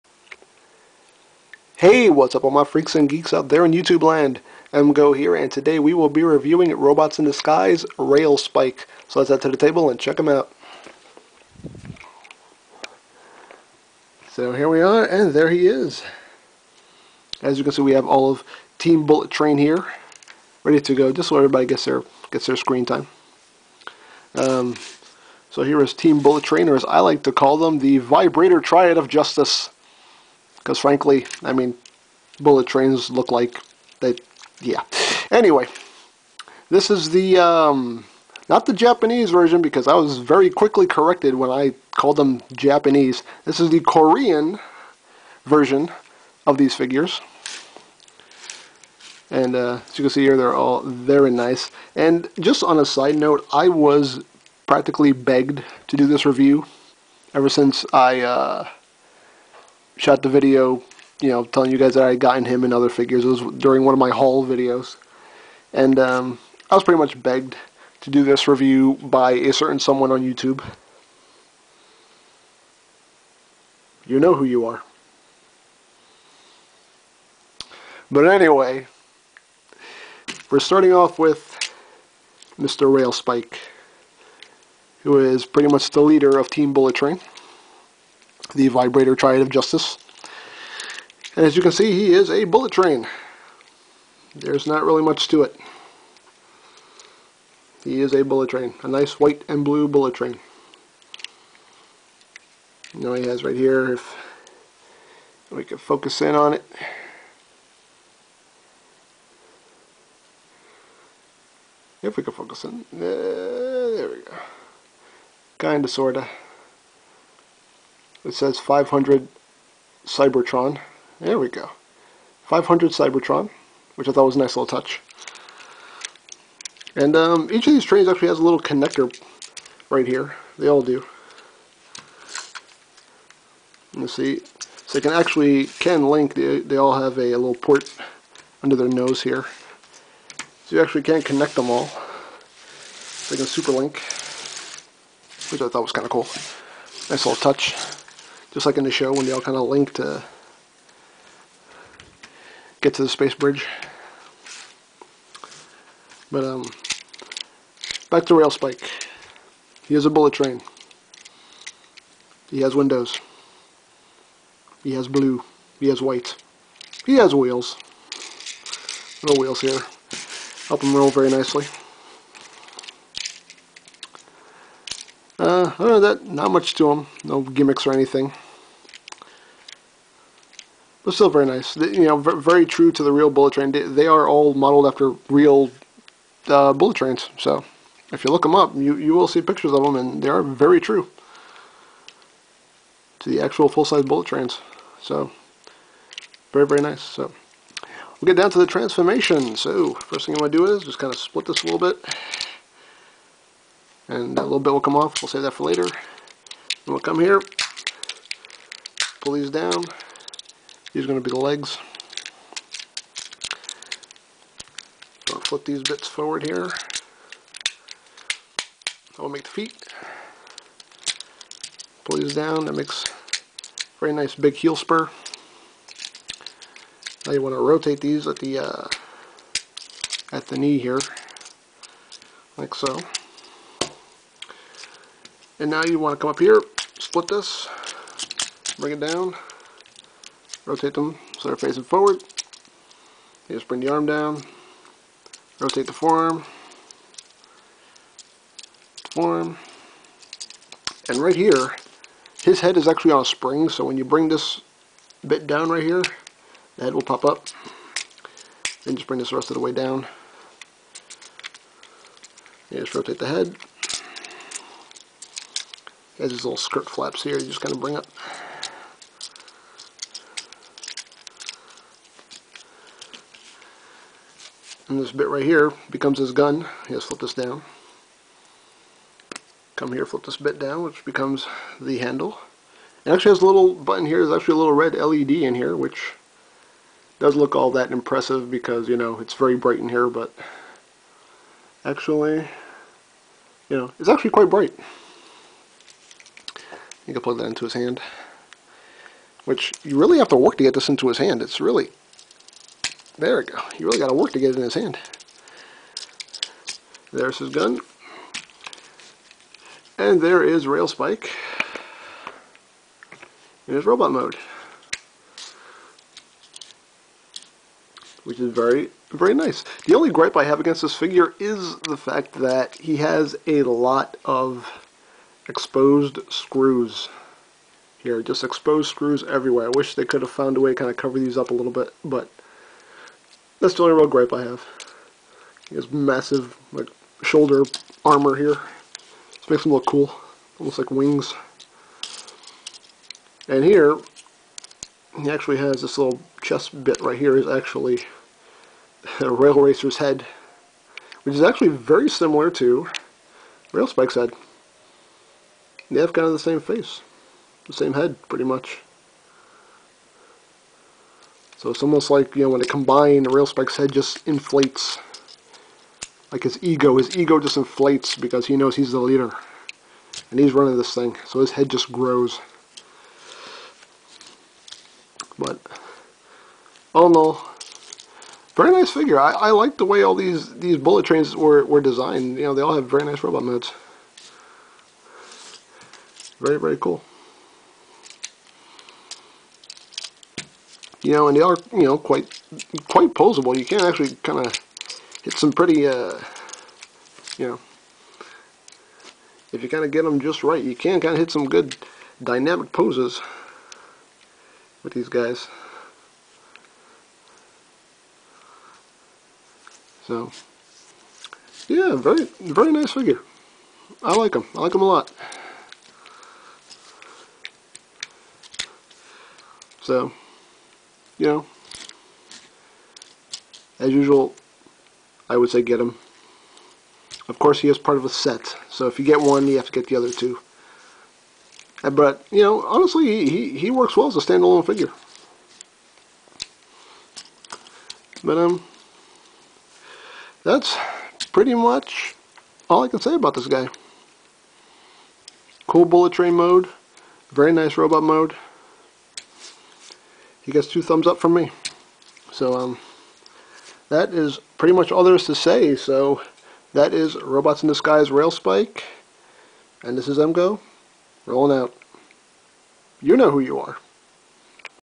Robots in Disguise RAILSPIKE: EmGo's Transformers Reviews 'N Stuff
Here is the first of team Bullet Train or as I like to call them, The Vibrator Triad of Justice. This set of reviews is going out to Lugnut1995 who's been requesting that I review these guys for a while. Happy viewing.